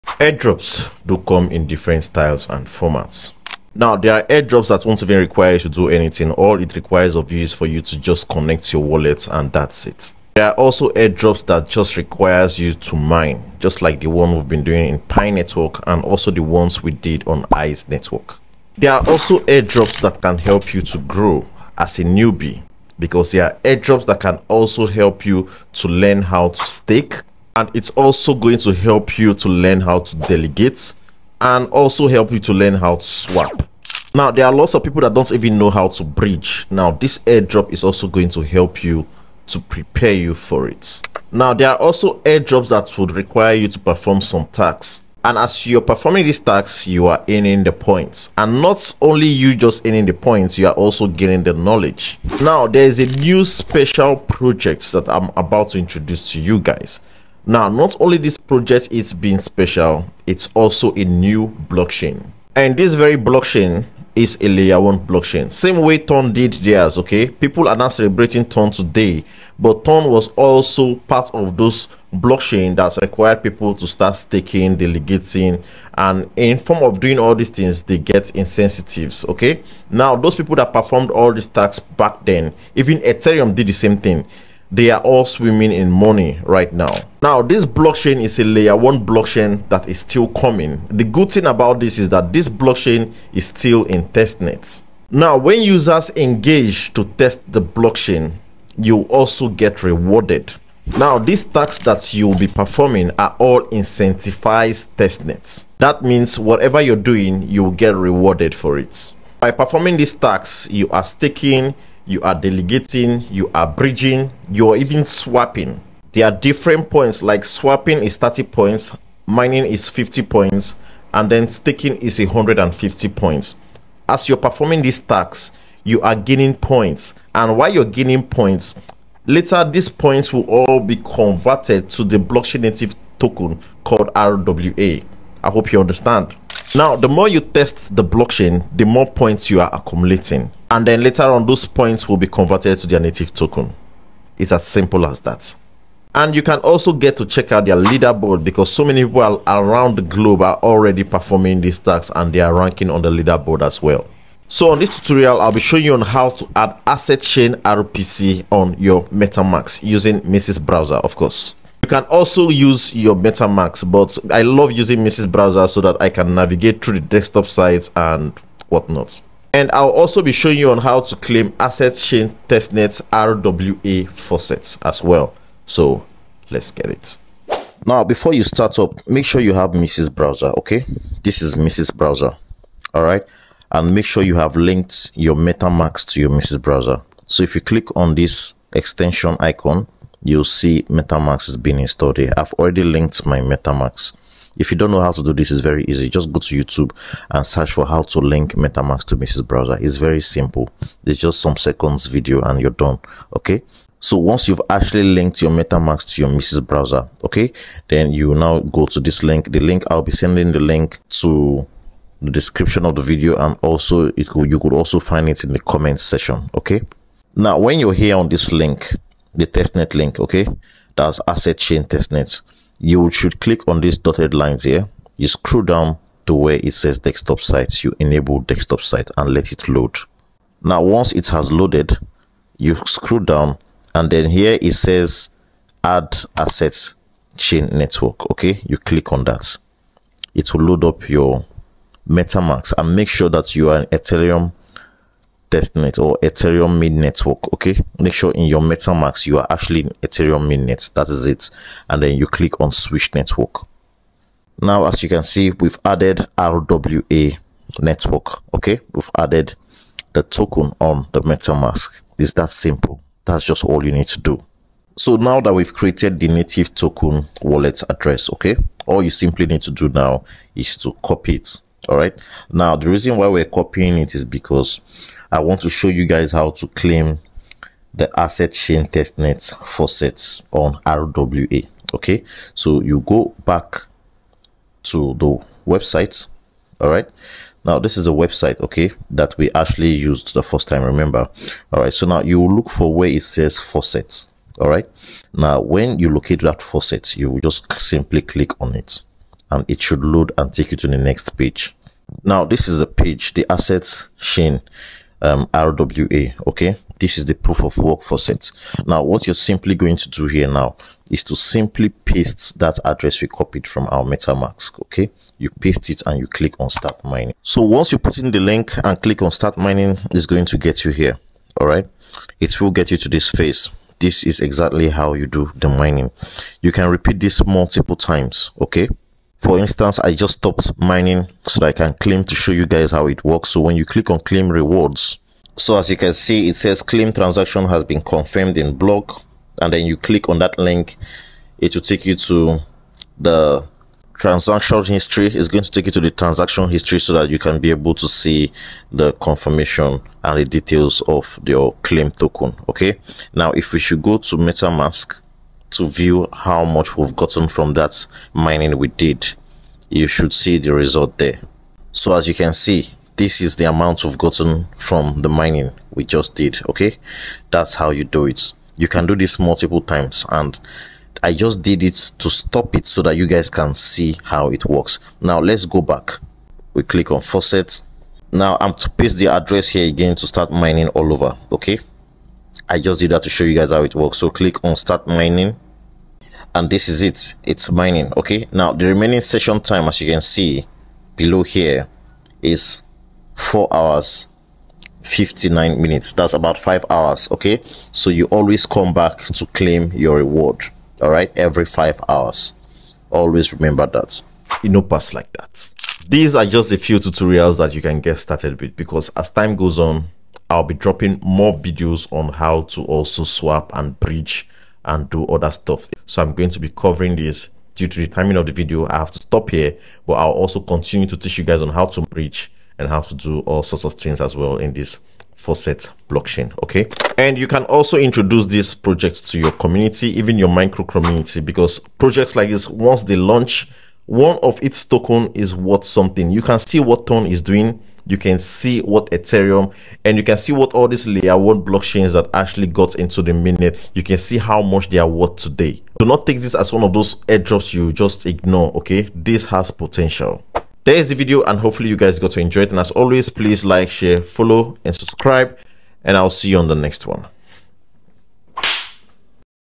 HOW TO ADD ASSET CHAIN TESTNET ON METAMASK || HOW TO MINE
*"Unlock Asset Chain Testnet Mining with MetaMask!*

In this step-by-step tutorial, learn how to add Asset Chain Testnet to your MetaMask wallet and start mining like a pro!

*Video Highlights:*

- Easy-to-follow instructions for adding Asset Chain Testnet to MetaMask
- Setting up your wallet for mining success
- Starting the mining process and earning rewards

- MetaMask wallet extension installed

*Watch & Learn:*

- How to access the Asset Chain Testnet network
- Tips for optimizing your mining experience

click on this link below to get started